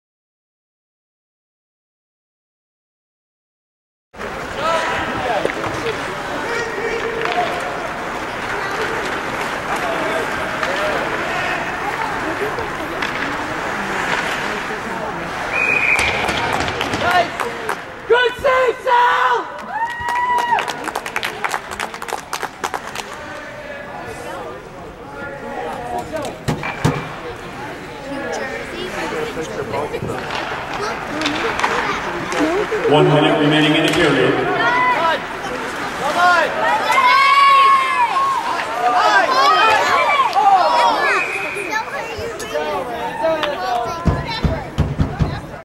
Rangers Pickup at MSG
Skating after the Rangers/Hurricanes Game at Madsion Square Garden